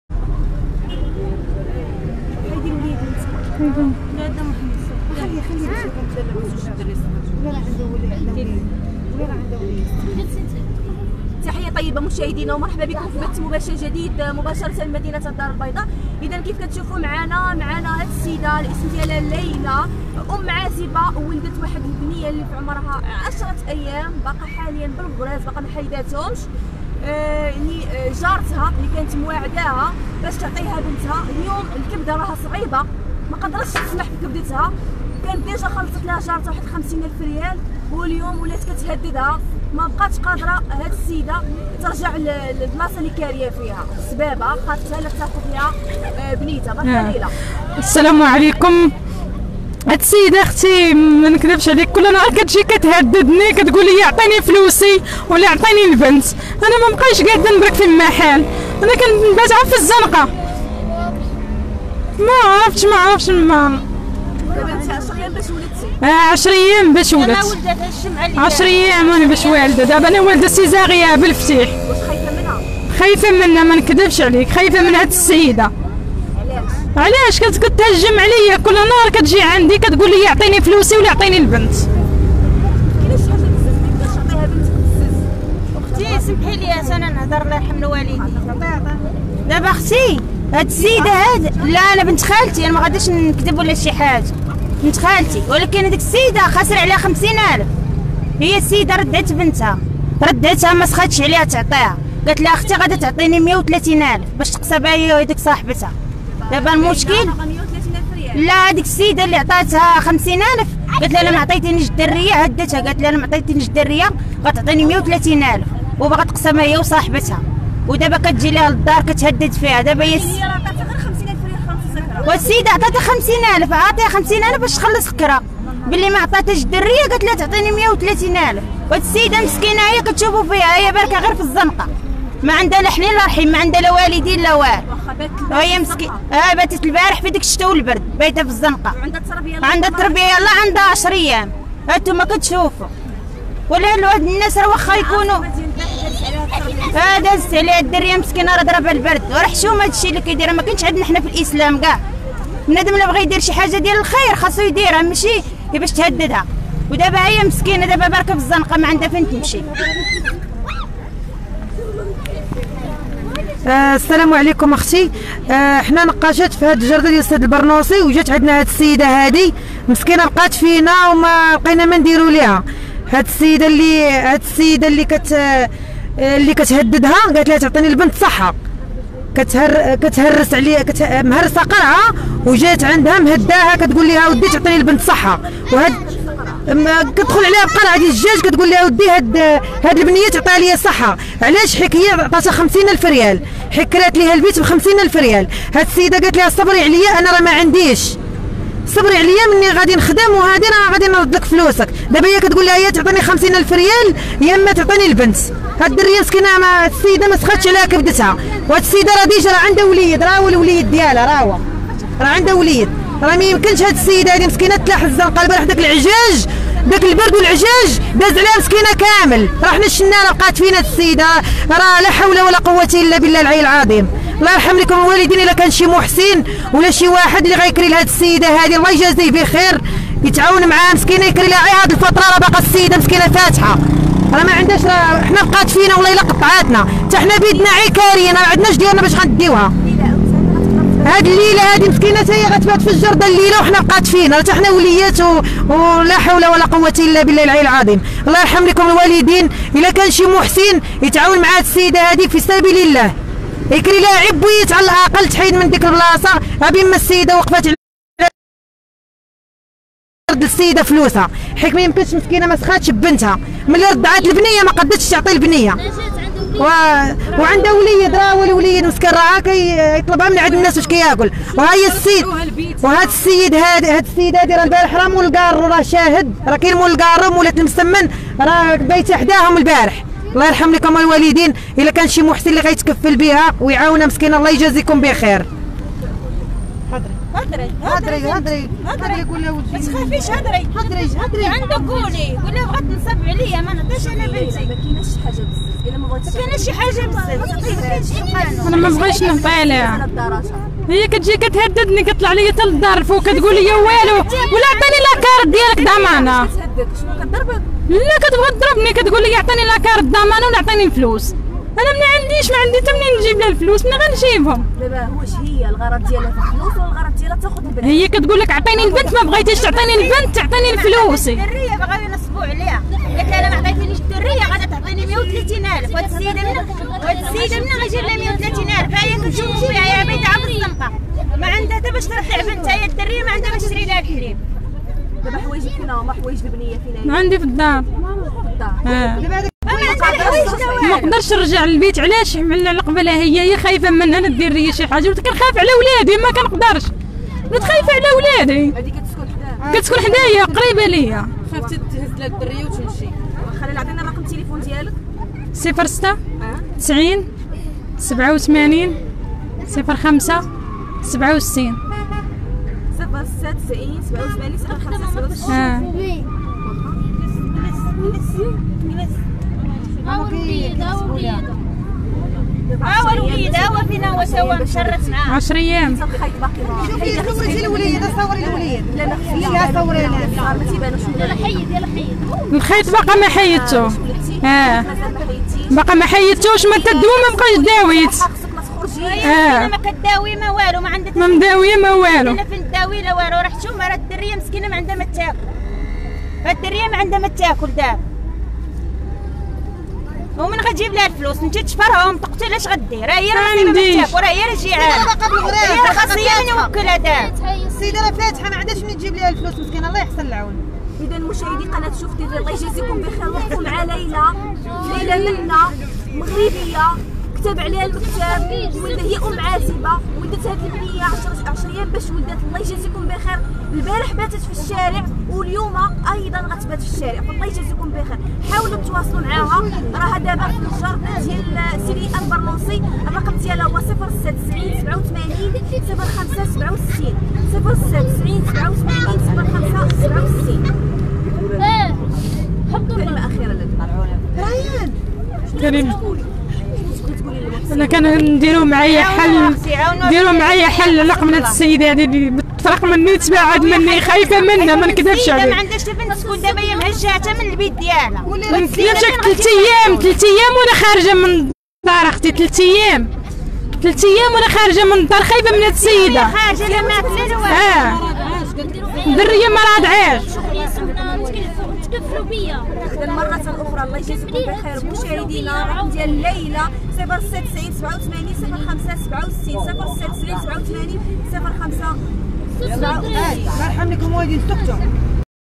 شابة حملات بلا زواج وولدات 10 أيام هاذي وكتبات فالزنقة وسيدة بغات تشريها من عندها ب50 الف ريال
مباشرة من الدار البيضاء..مشهد صادم: شابة حملات بلا زواج وولدات بالفتيح غير 10 أيام هاذي ودابا كتبات فالزنقة وحالتها خطيرة وصادمة وواحد السيدة بغات تشري من عندها الرضيع بـ50 ألف ريال